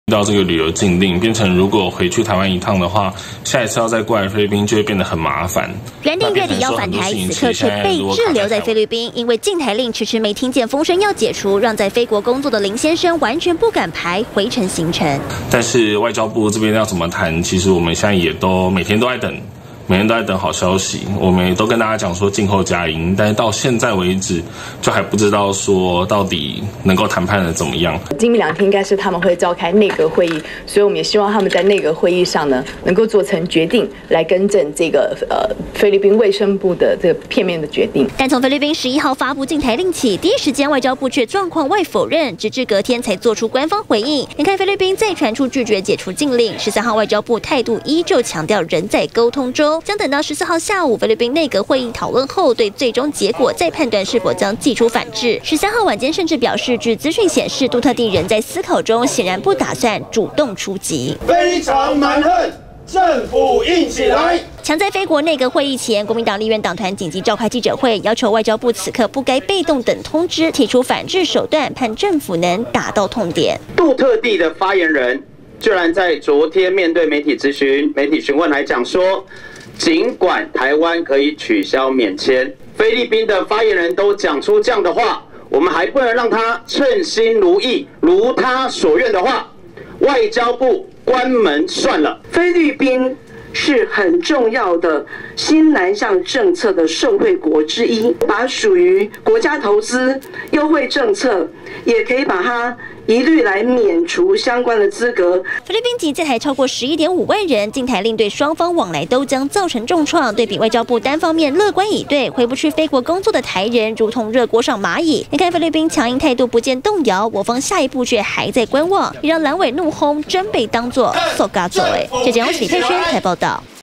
20200214中天新聞 菲律賓禁台令拒解除 外交部態度軟挨轟
菲律賓禁台令，外交部始終沒表達，強硬態度反制，還說要等到今天，菲國內閣會議後，再做出因應決定，藍委怒轟，外交部，乾脆關門算了，滯留菲國工作的台人也坦言，不敢出國，很怕回台後，無法返回菲國工作。